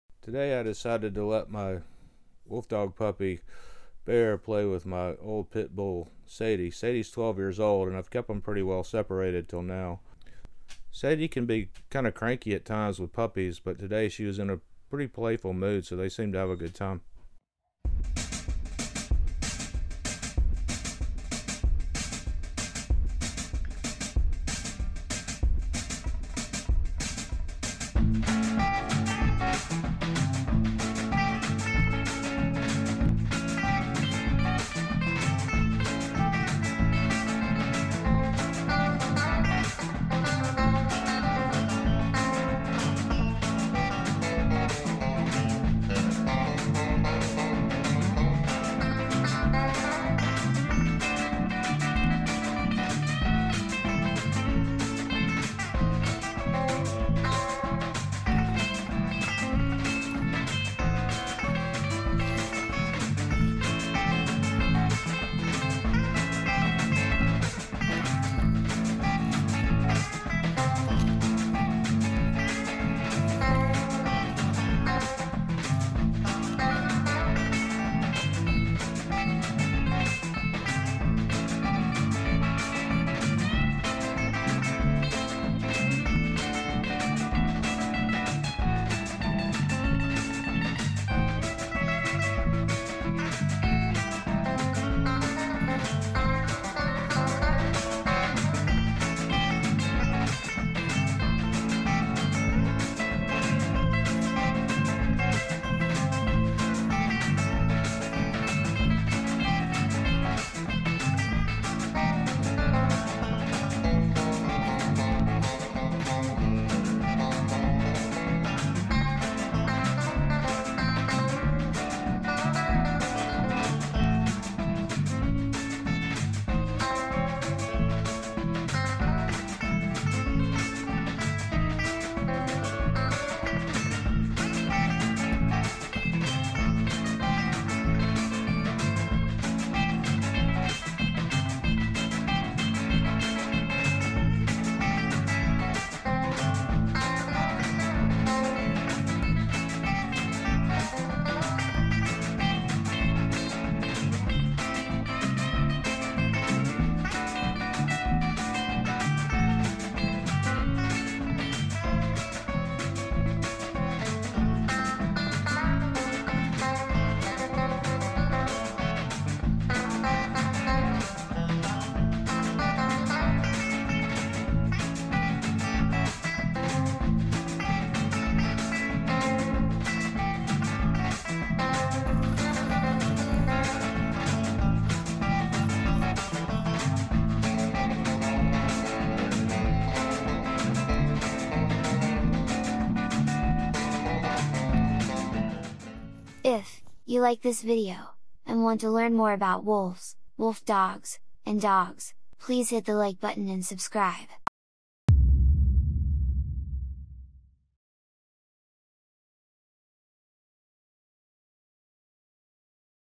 Wolf Dog Puppy Meets Old Cranky Pitbull And Here is What Happened
Bear my wolf dog puppy playing with my 12 year old pit-bull mix Sadie. He is on a lose leash which gives them both the means to run around, Sadie can get cranky with puppies if inside cause she cant get away from them, Outside is another story. Wolf dogs are cool but please do your research and learn ahead of time before you get one. They are not right for most people